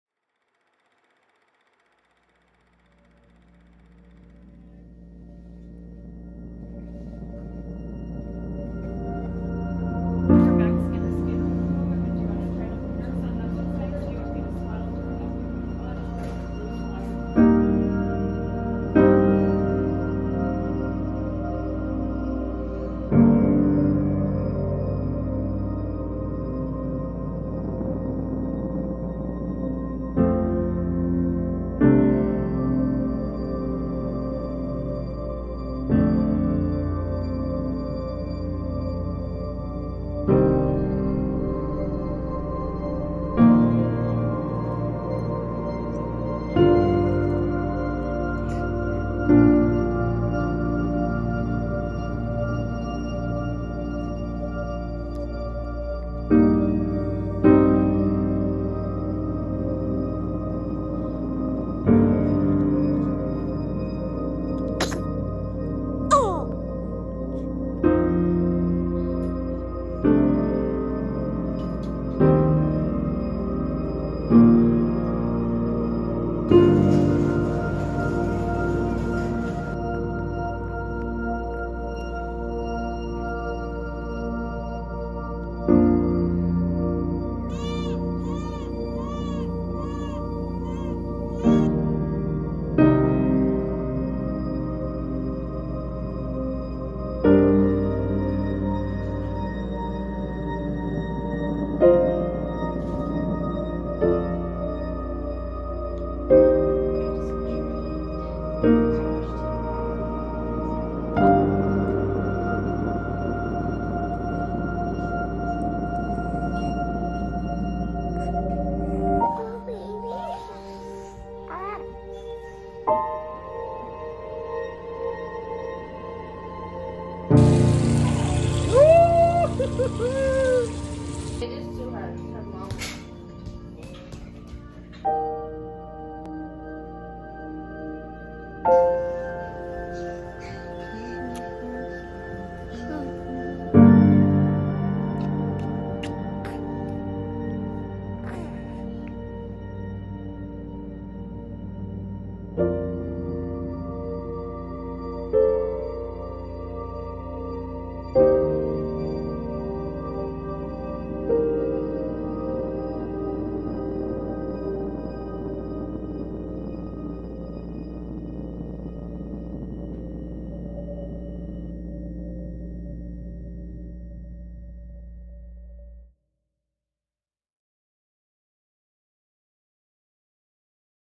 Norah’s first month!
Tried to take a video everyday for Norah’s first month. Hope this will be something that she’ll look back on.